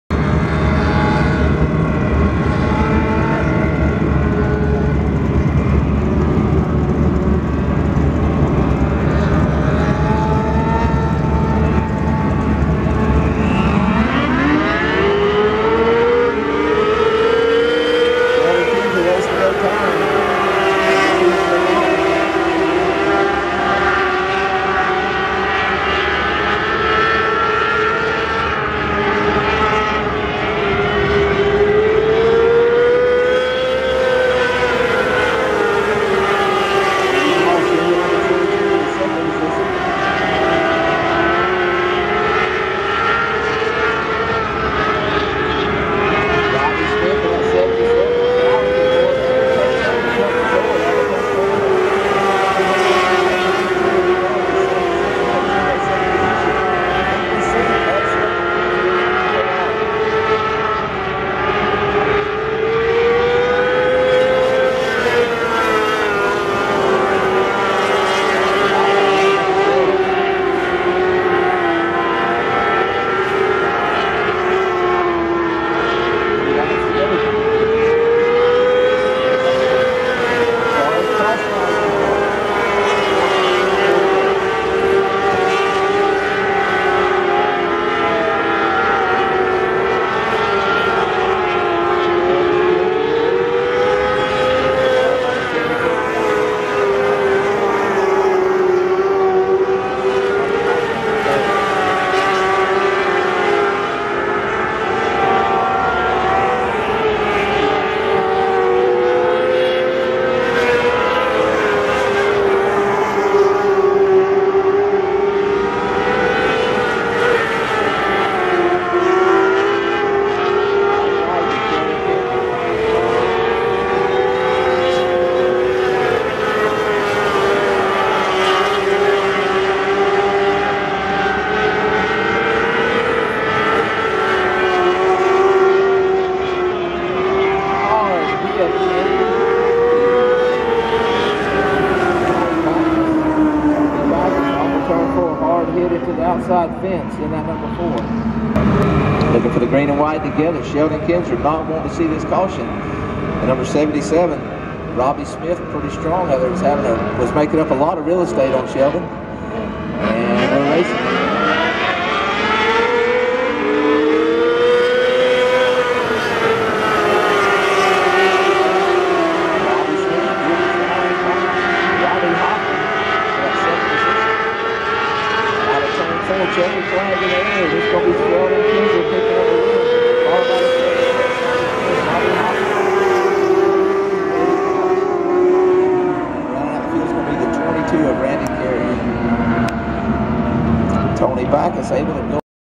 Racing | FMSA Mini Sprints | Heat Race #1 | Bubba Raceway Park | 12-13-14
Bubba Raceway Park
FMSA Mini Sprints  Heat Race  #1

Make Sure To Tell Your Friends And Family To Do So As Well So They Can Watch The Races Too. Please 'LIKE' And 'SHARE' My Videos On YouTube Thanks Everybody!!